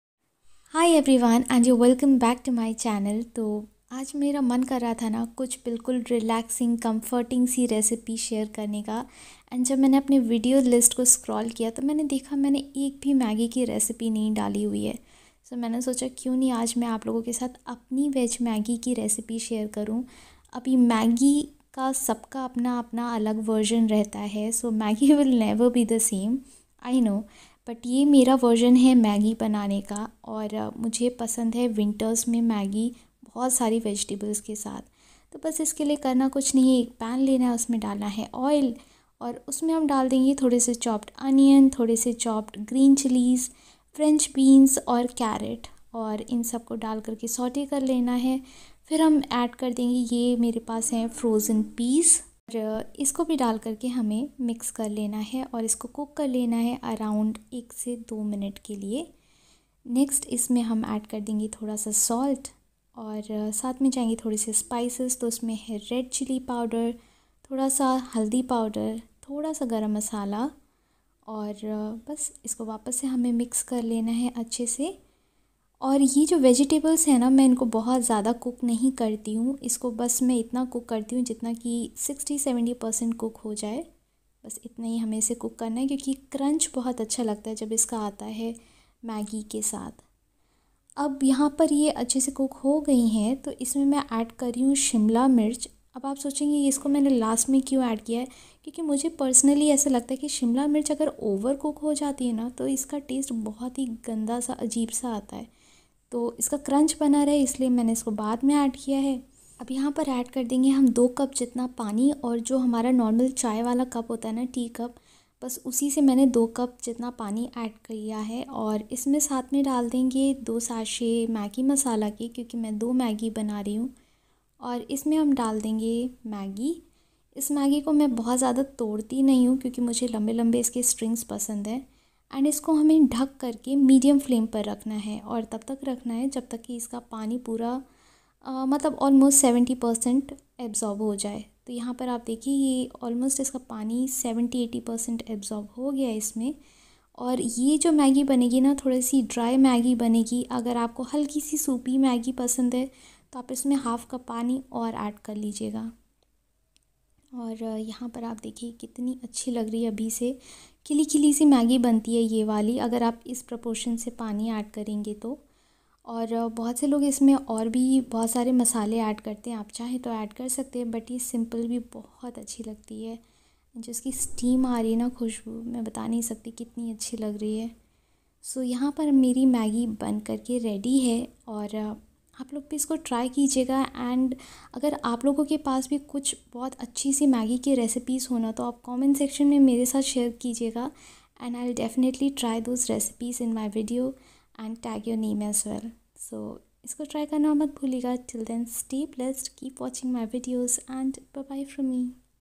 Maggie masala recipe। Maggie recipe।Veg Maggie recipe। मैगी रेसिपी in हिन्दी
Hey everyone,Today I am going to make very easy veg Maggi, kindly follow all the steps as shown in video. @Srishti Srivastava